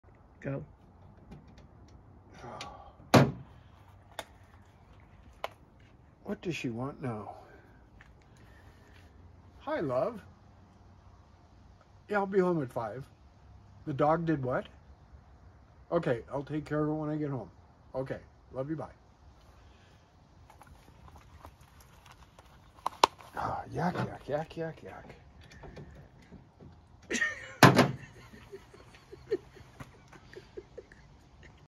Bison Cell-Phone Holster
The design of this bison phone holster is the most comfortable and easiest to wear. Textured bison leather is handcrafted in Montana to our specifications. A faux nickel bison snap keeps the celly-telly safe, a finger slot allows for pushing phone up into the hand instead of having to dig for it. Holster fits most Android and iPhones with a case, measures 7X4X1 / accommodates a 1.5" wide belt.